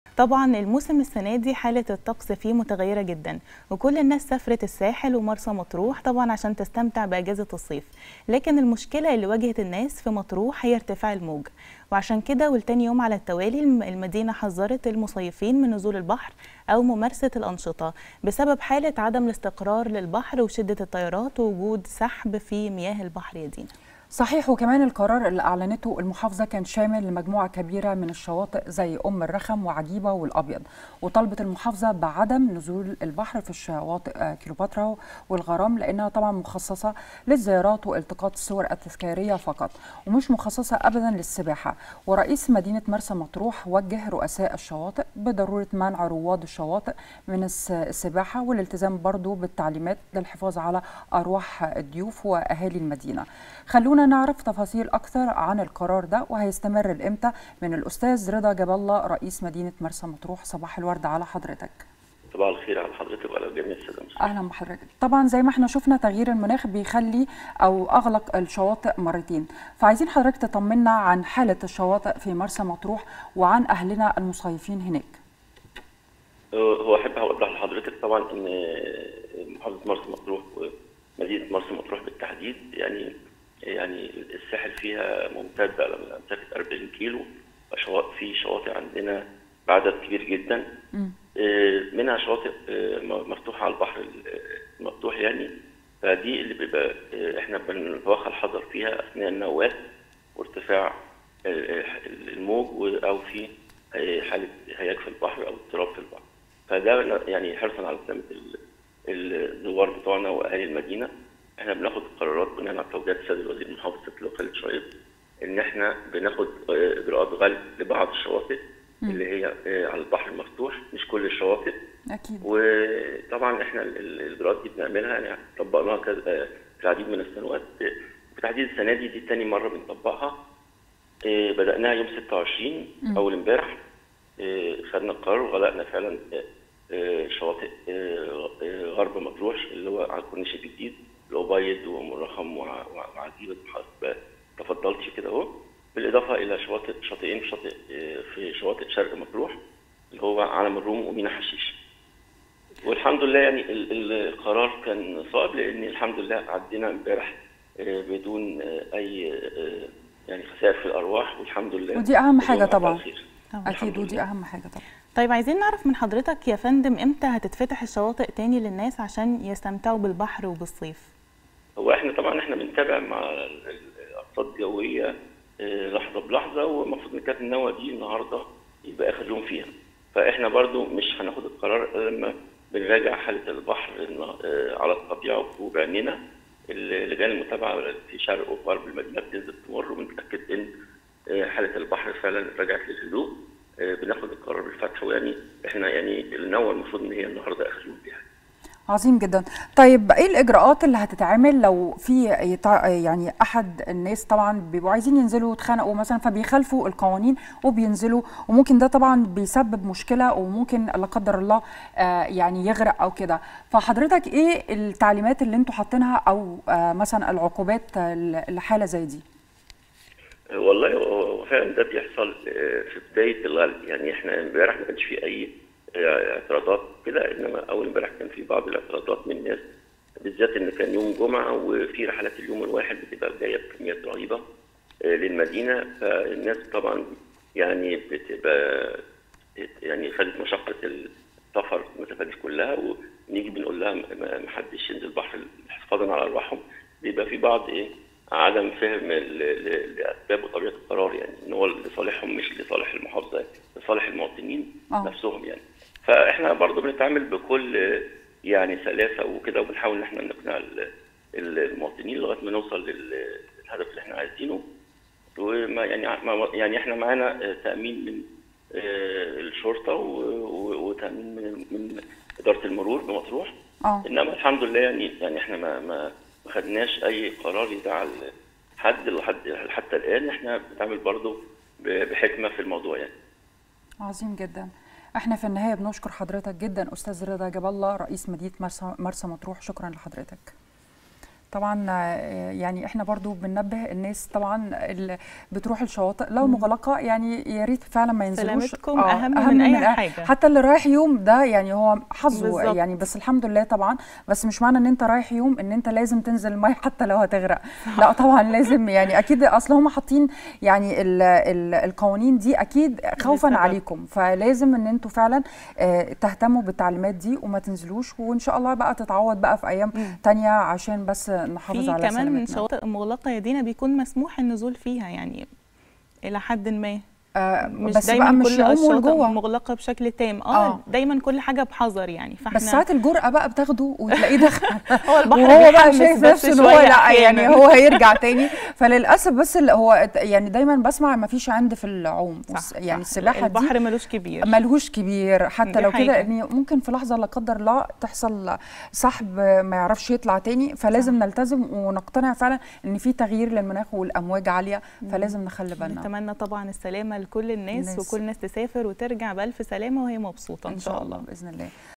رئيس مدينة مطروح: قرارات غلق الشواطئ تتم بعد مراجعة حالة البحر للحفاظ على حياة المواطنين
صباح_الورد .. قال رضا جاب الله رئيس مدينة مرسى مطروح، إن ساحل المدينة يمتد لأكثر من 40 كيلو منها شواطئ مفتوحة وننصح المصيفين بتوخي الحذر أثناء التواجد على الشواطئ، ونتخذ إجراءات غلق لبعض تلك الشواطئ للحفاظ على المتواجدين، وبدأن تنفيذ غلق بعض تلك الشواطئ.